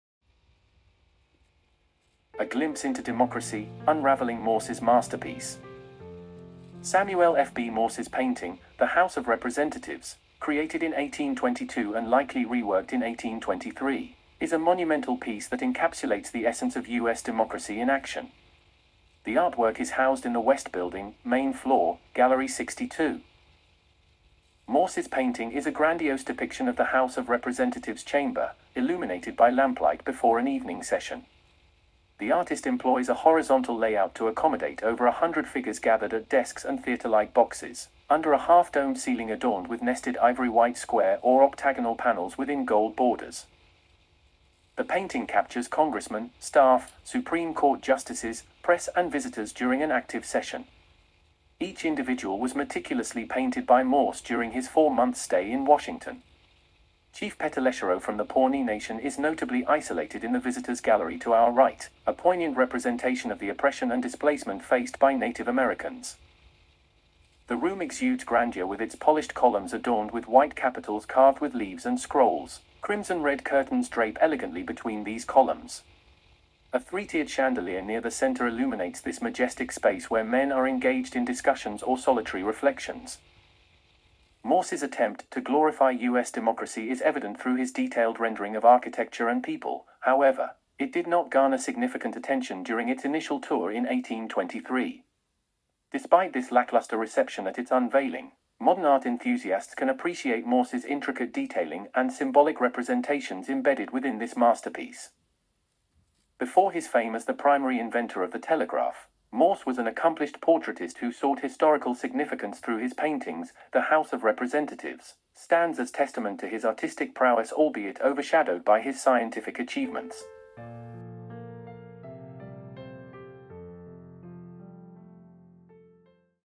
A Glimpse into Democracy: Unraveling Morse’s Masterpiece, The House of Representatives, 1822
With this monumental painting, Samuel F. B. Morse wanted to glorify US democracy in action. He depicts the grand House of Representatives chamber glowing in lamplight before an evening session.

HouseofRepresentatives, #1822 House of Representatives, #1822, probably reworked #1823 Samuel F. B.